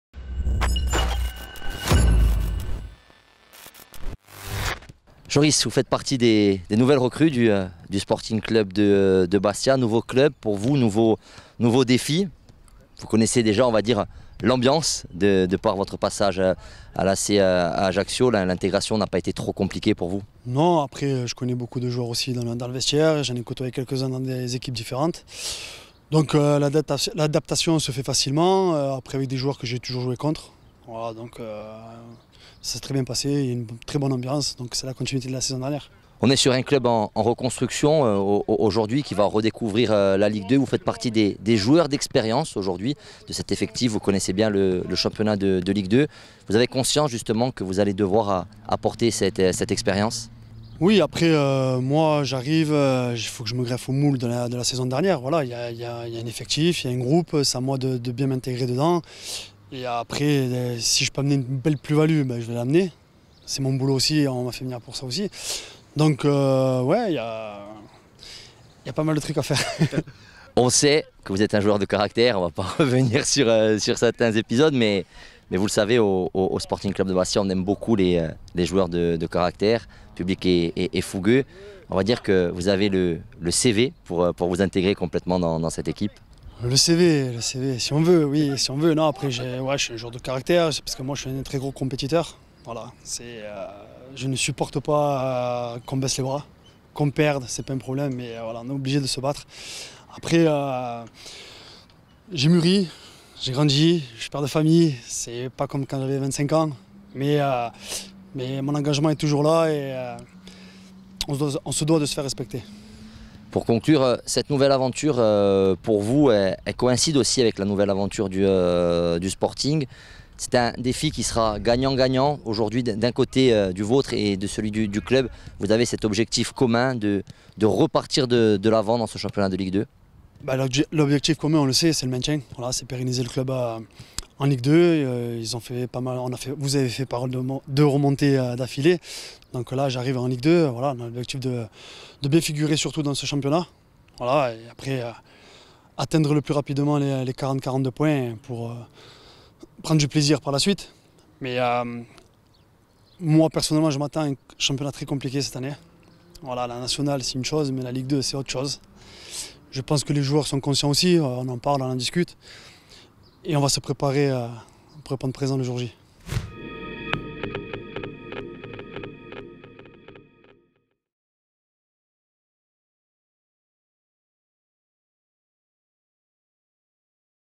Joris Sainati🎙 : « On va bien se préparer pour répondre présent le jour J »
Retrouvez les premiers mots du nouveau défenseur bastiais Joris Sainati.